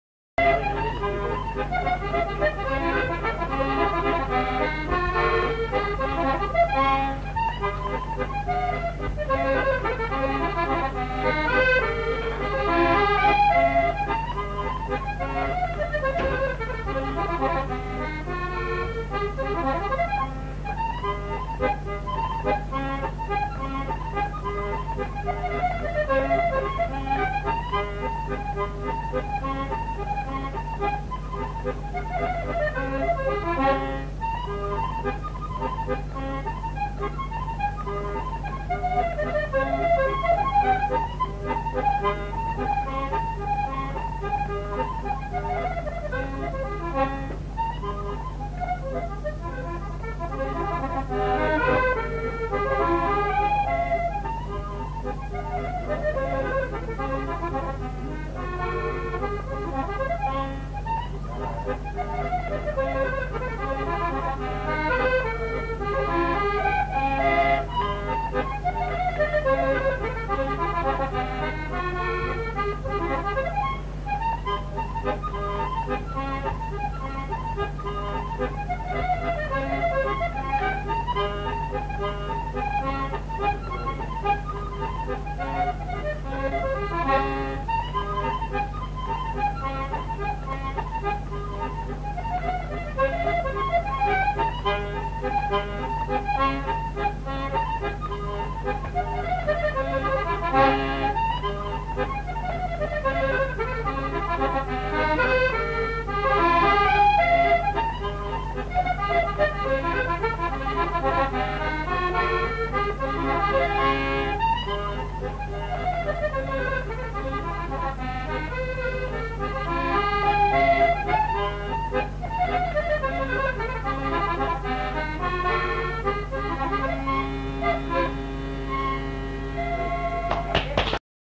Finbarr Dwyer's Waltzes 1980 rare recording.
Rare Recording of Finbarr Dwyer playing in The Man Of Aran Pub Dublin in 1980.